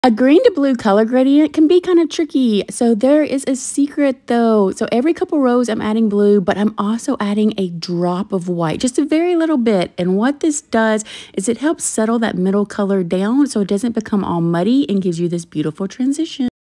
How to paint a green to blue color gradient
How to paint a color gradient of green to blue.  Heart Dot art acrylic painting.